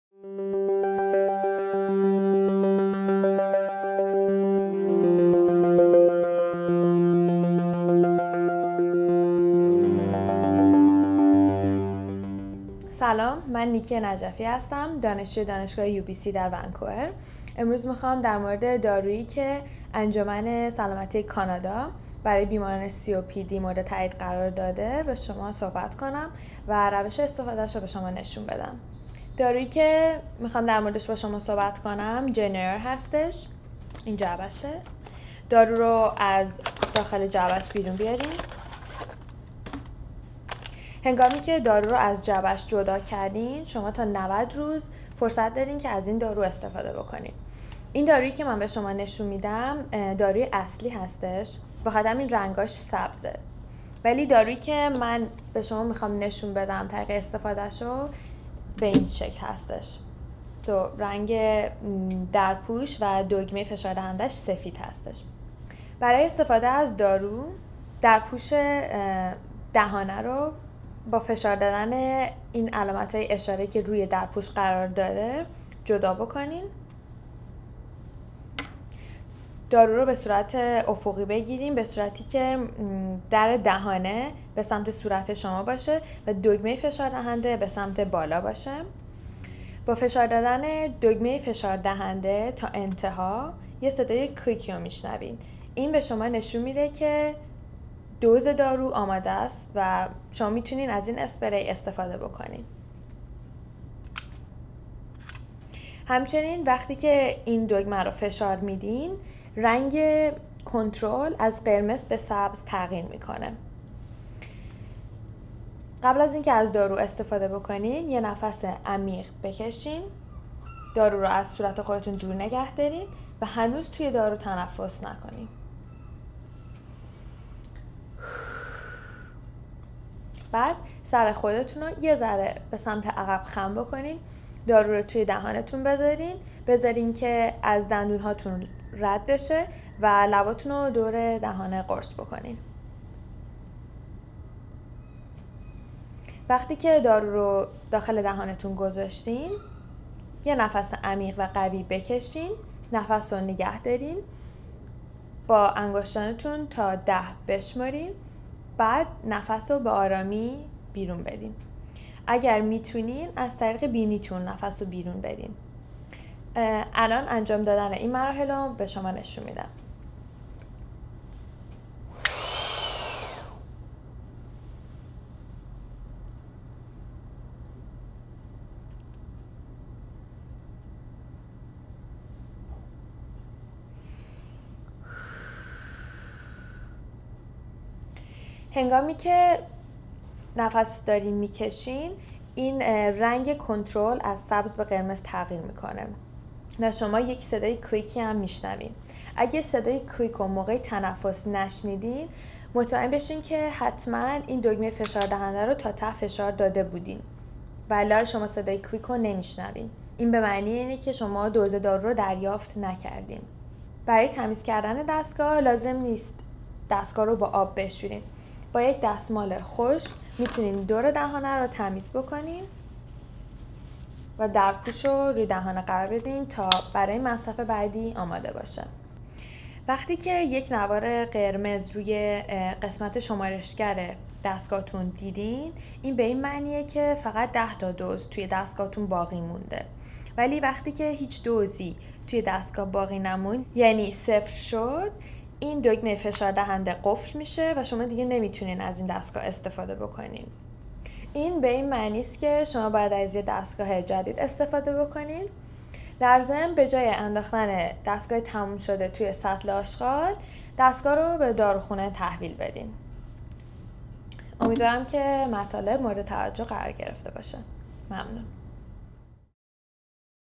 COPD Inhaler Techniques Video Farsi 3 Genuair
Farsi-speaking health care professionals were involved in the development of culturally sensitive and linguistically relevant educational materials for COPD patients in Vancouver, Canada. The aim is was to educate COPD patients from ethno-cultural communities to correctly use their inhalers and engage them in self-management of their chronic illness by means of improving their access to relevant information. The video has been developed by the research team at University of British Columbia’s Institute for Heart and Lung Health with financial and technical support provided by BC Lung Association and Vancouver Coastal Health Authority, Integrated Primary and Community Care.